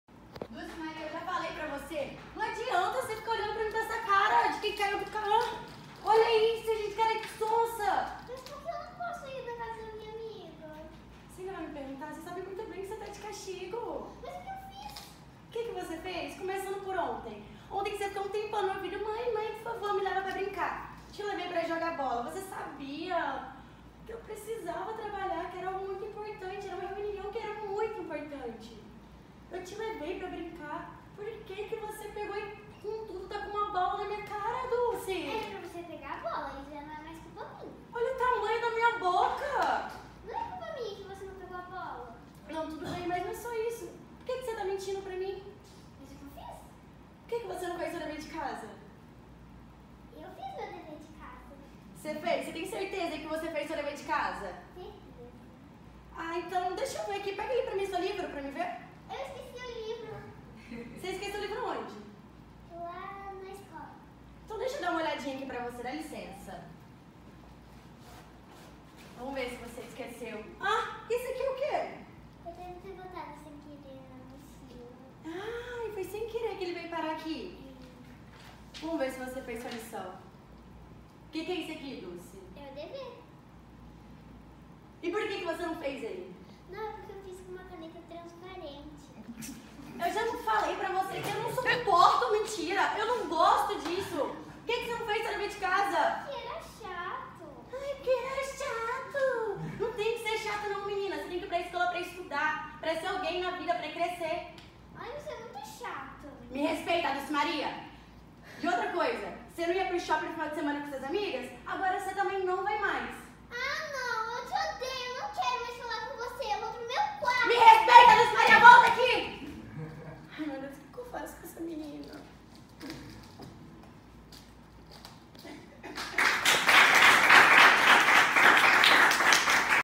Tira essa Cara de Sonsa
Dulce Maria quer ir brincar na casa das amigas mas mentiu para sua mãe Isadora e não estava fazendo o dever de casa. Será que a Mãe de Dulce vai deixar esta mentira passar?